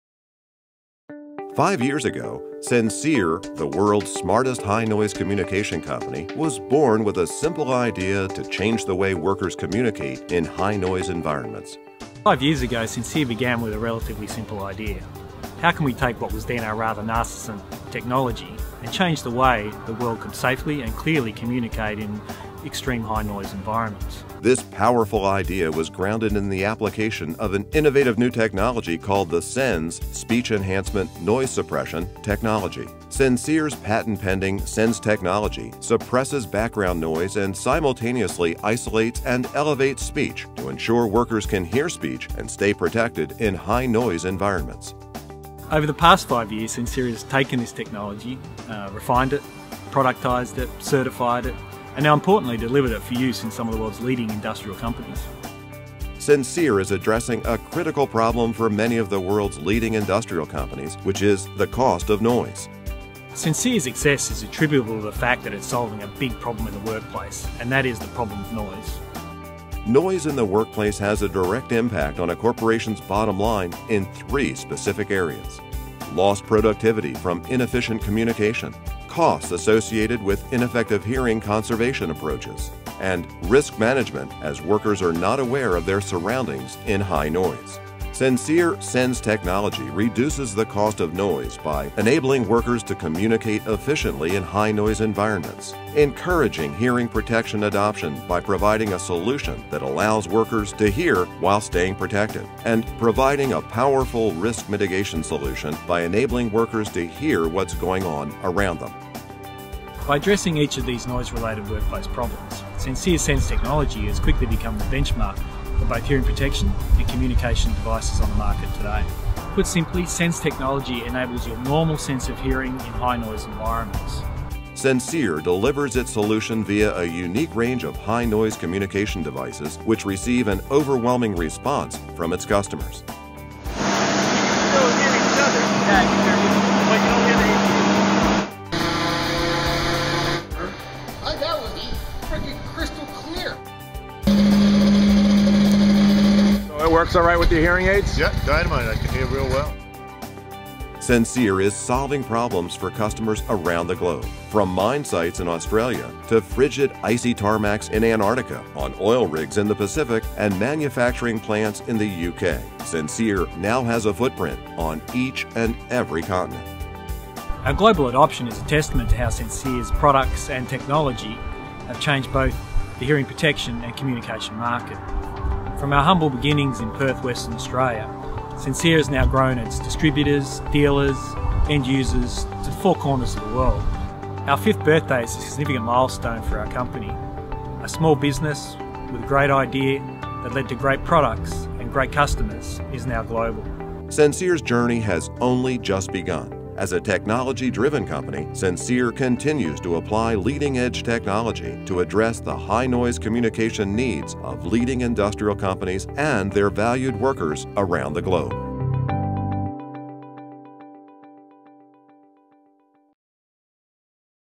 Sensear 5 Year Anniversary
Five years ago Sensear began with a simple idea to change the way people communicate in high noise. Now we're a global leader. Watch our story.